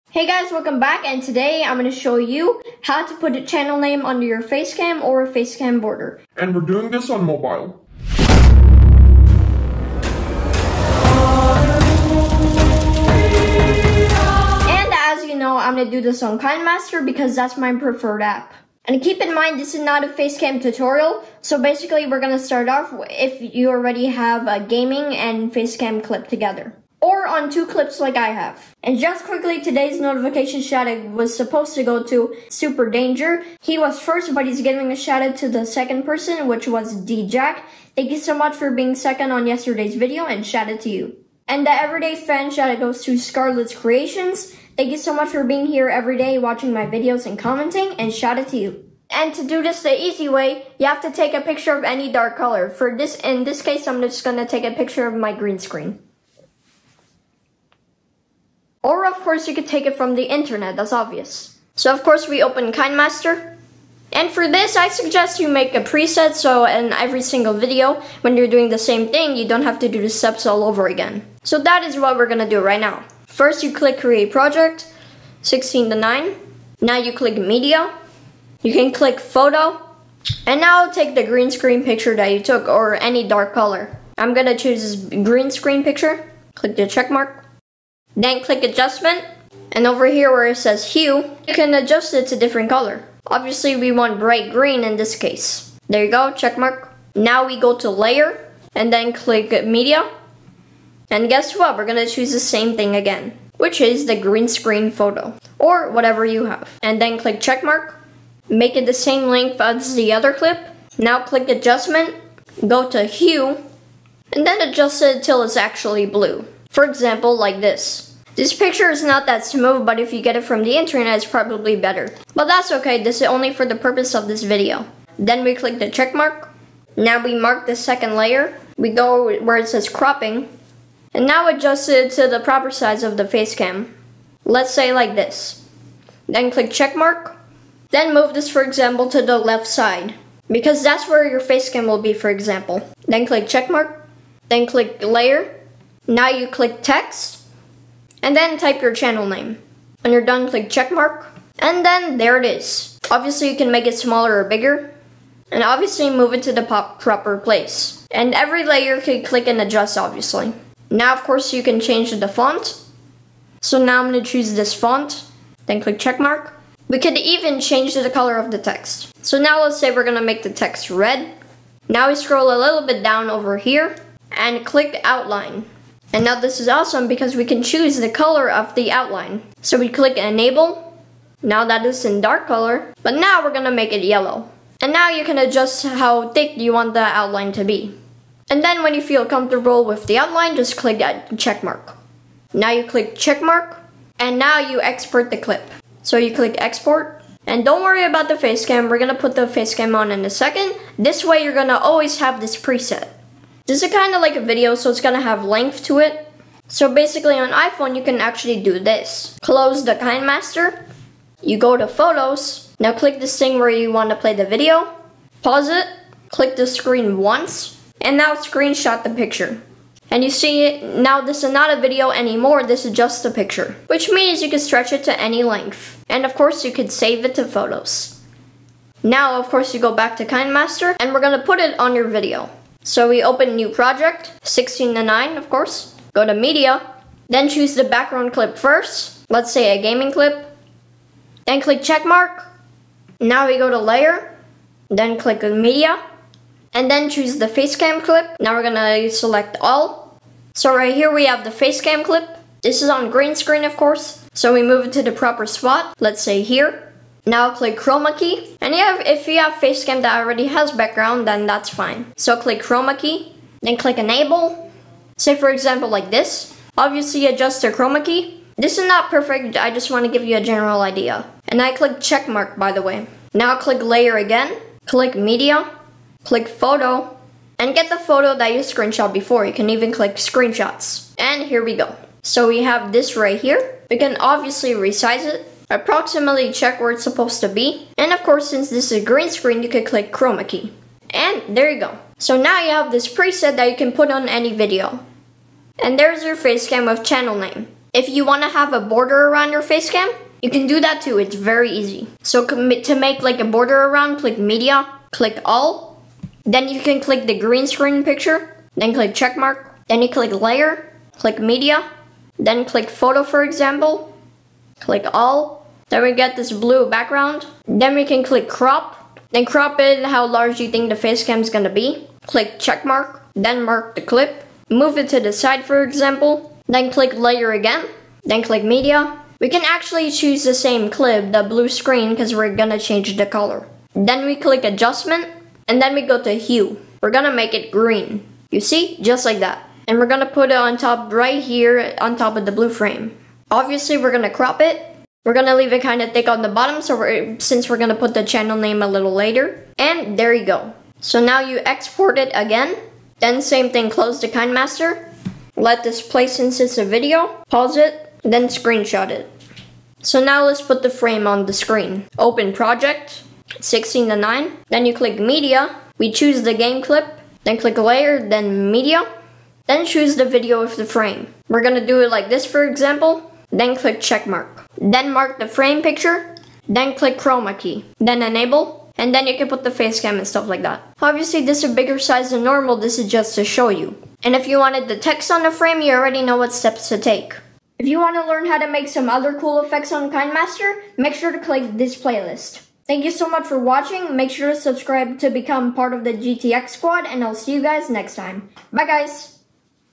Channel Name Under Face Cam tutorial - Channel Name on Face Cam Mobile tutorial
Channel Name Under Face Cam tutorial | ChrisGTXvideos
So You have great Video and Wanna Put a Face cam with Your channel name Under throughout the Video . All You need to do to have the channel Name on Frame or Border is to have some green screen Picture or some similar Colors .
In This Video i'll show You How to put Channel name Under Your Face cam , Border and Even How to Make it As a Preset So Yo'll Always Have The Channel Name frame Ready to use for Your Next Videos .

Putting Channel name on face cam is Super easy but it takes few steps.
So Open KineMaster , Go to Media and Choose Your Green Screen picture , Make it Full Screen.
Then Go to Layers in kineMaster and You can actually select that Clip again From The Media as a second Layer . Now Highlight the 2nd. layer Clip and click Cropping on The Right side , Then crop it to The Bar Size that would be Under the Face cam .
Now also on The right side Click Adjust and change the HUE aka Color to Different color , let's say Blue.
Now go to Layer and select Text , Put Your Channel Name Under where the face cam would be , select Font , Color .Then on scroll down and You can click outline and select Different Color for Your channel Name Text.
Adjust it to Proper size and Here is is Your face cam with Channel Name .